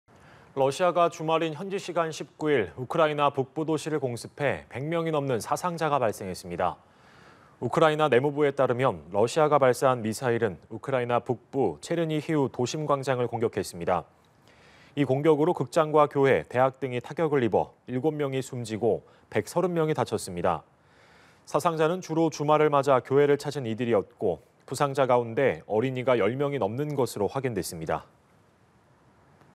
주말 우크라 북부 도심에 러 미사일…130여명 사상 / 연합뉴스TV (YonhapnewsTV)
주말 우크라 북부 도심에 러 미사일…130여명 사상

러시아가 주말인 현지시간 19일 우크라이나 북부 도시를 공습해 100명이 넘는 사상자가 발생했습니다.

우크라이나 내무부에 따르면 러시아가 발사한 미사일은 우크라이나 북부 체르니히우 도심 광장을 공격했습니다.

이 공격으로 극장과 교회, 대학 등이 타격을 입어 7명이 숨지고 130명이 다쳤습니다.

사상자는 주로 주말을 맞아 교회를 찾은 이들이었고, 부상자 가운데 어린이가 10명이 넘는 것으로 확인됐습니다.